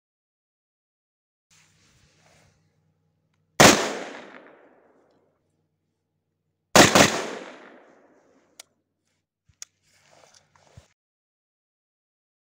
V-AR kompenzátor vs. flash-hider | NoCompromise.House
Porovnání chování kompenzátoru a flash-hideru českého výrobce V-AR (Pavla Vosátky).